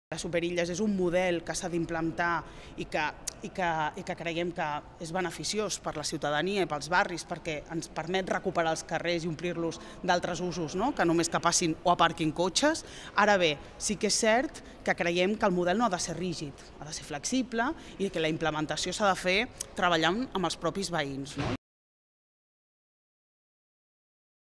Janet Sanz: "El model de superilles ha de ser flexible i treballar-lo amb el veïnat"
(19/12/2016) El model Superilles de l’Ajuntament de Barcelona pretén redefinir l’espai públic de la ciutat com a llocs habitables, espais comunitaris, territoris d’extensió de la residència i àmbits per al joc, el verd, la història i la vida local dels barris. Per fer-ho planteja repensar la ciutat a partir de noves formes d’agrupació, basades en les illes tradicionals, replantejant el rol dels carrers i fomentant la interacció i l’agrupament social.